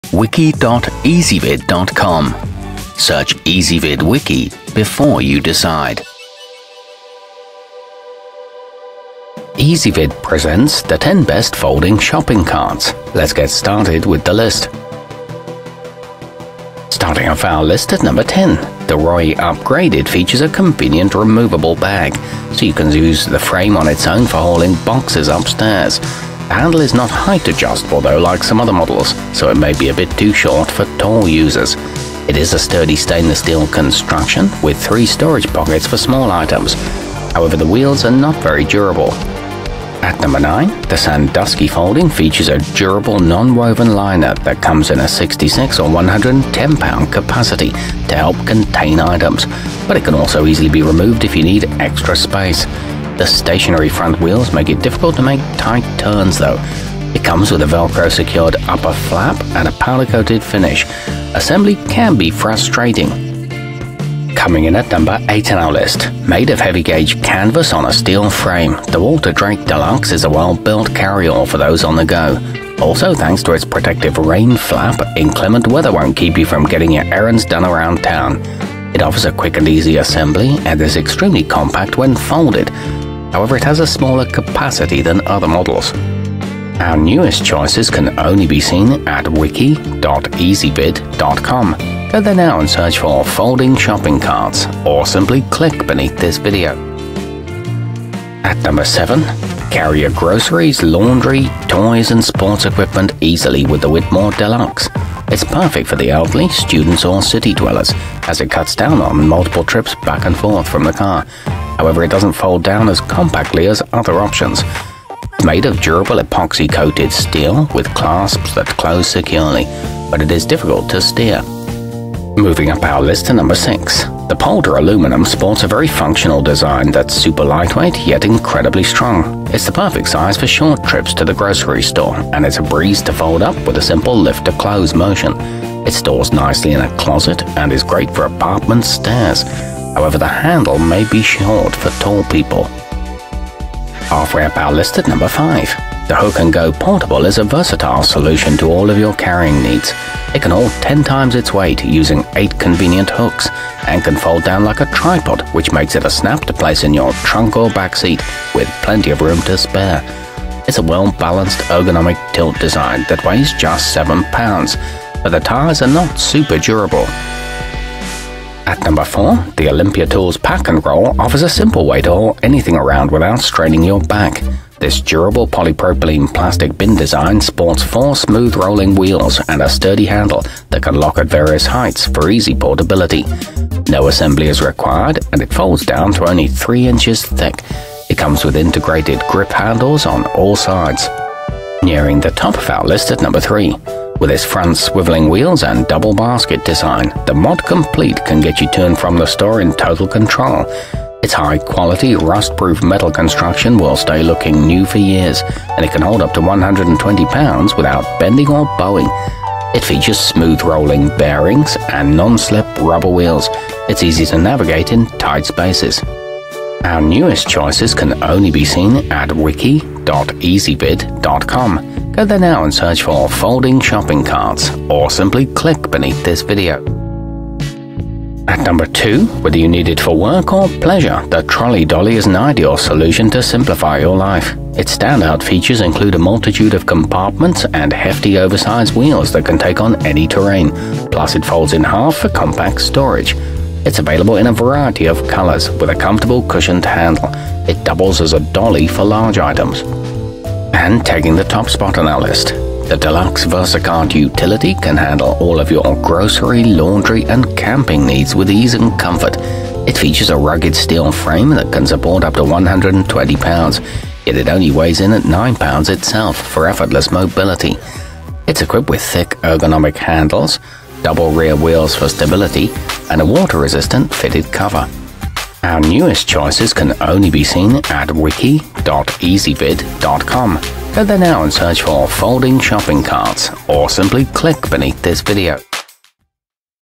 10 Best Folding Shopping Carts 2018
Our complete review, including our selection for the year's best folding shopping cart, is exclusively available on Ezvid Wiki.

Folding shopping carts included in this wiki include the whitmor deluxe, royi upgraded, mod complete, trolley dolly, olympia tools pack-n-roll, versacart utility, polder aluminum, walterdrake deluxe, hook and go portable, and sandusky folding.